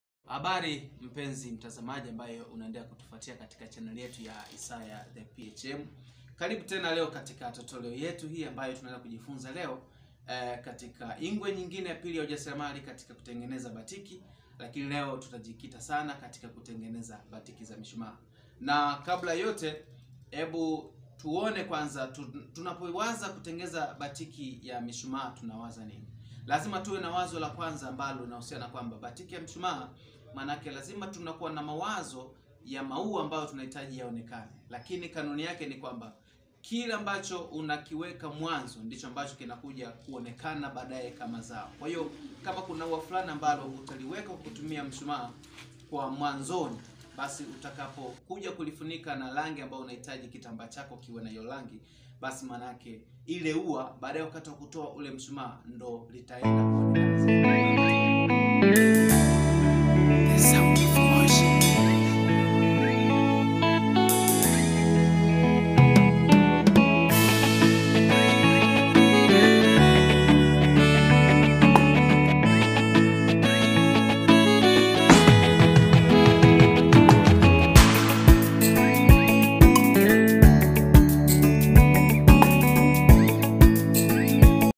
BATIKI YA MSHUMAA (DARASA)
Karibu Katika ujasiliamali. DARASA litaanza hivi punde na tutajifunza Utangenezaji wa BATIKI Aina zote yaani MSHUMAA, Tie and Die na brich.